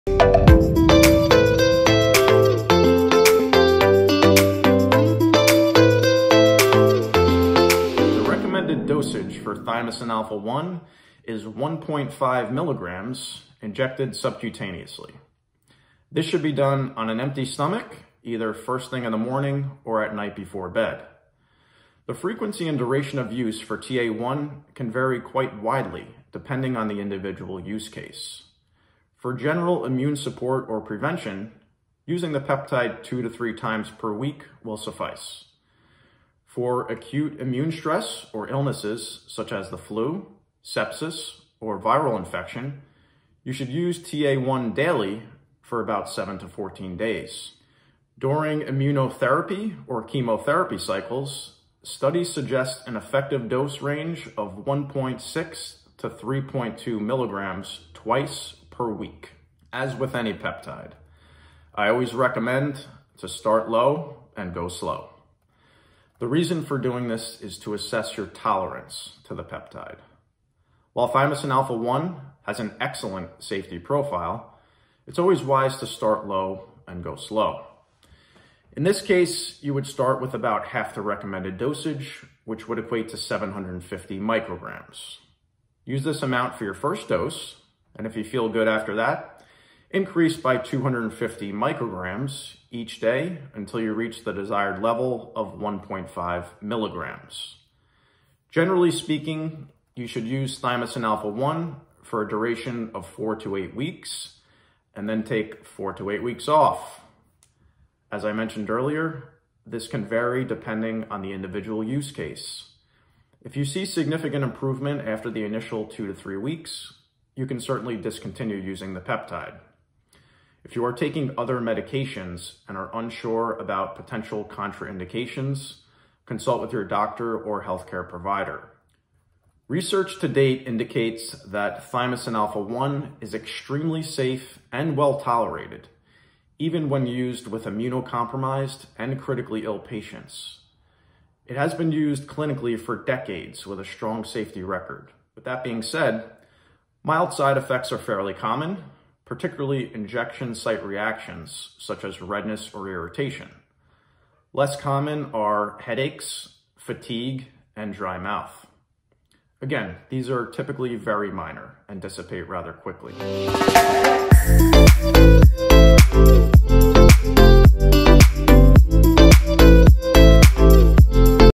How to Use Thymosin alpha-1 Peptide | Dosage, Timing, and Usage Guidelines
Learn the essentials for using Thymosin alpha-1 (TA-1): recommended dosage, timing, and duration of use for this immune system peptide.